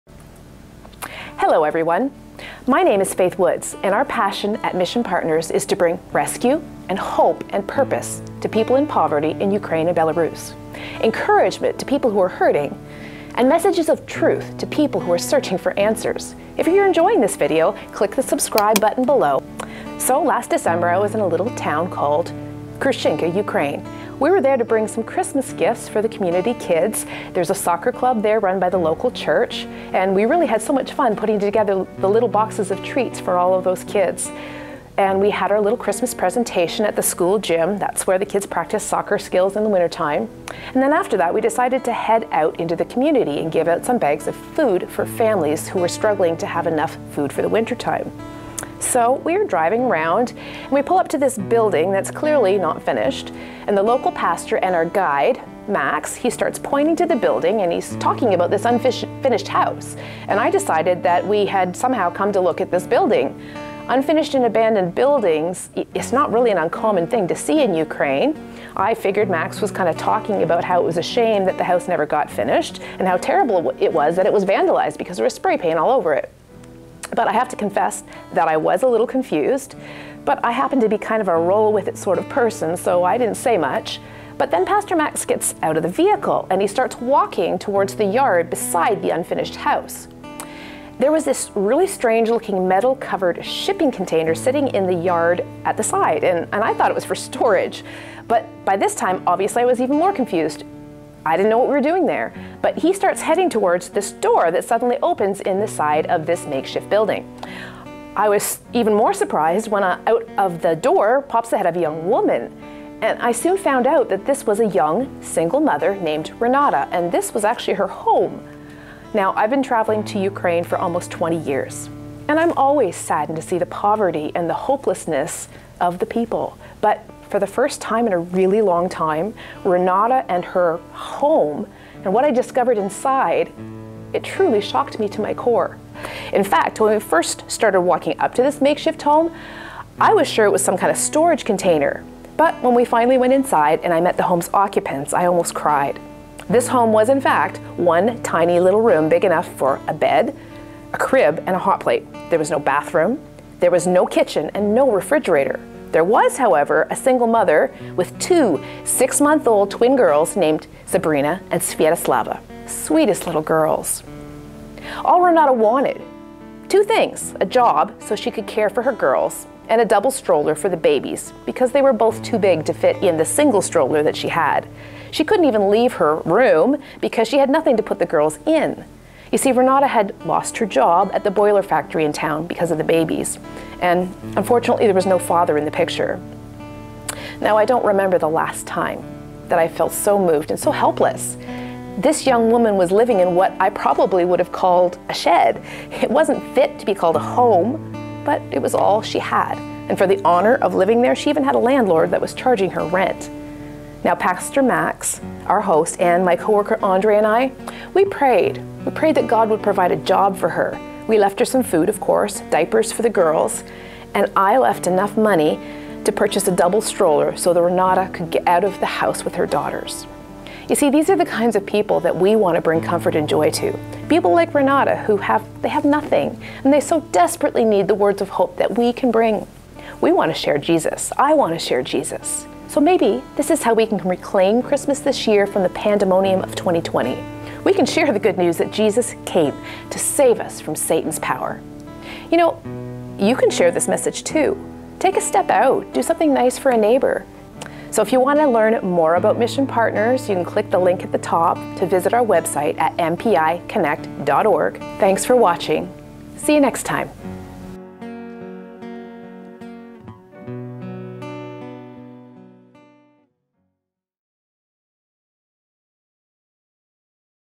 Life Story: Poverty in Rural Ukraine
This young single mother, Renata, lives with her twin daughters in what looks like a storage shed. Her story is only one example of the rising poverty in rural Ukraine today. Mission Partners is doing whatever we can to help families like Renata's.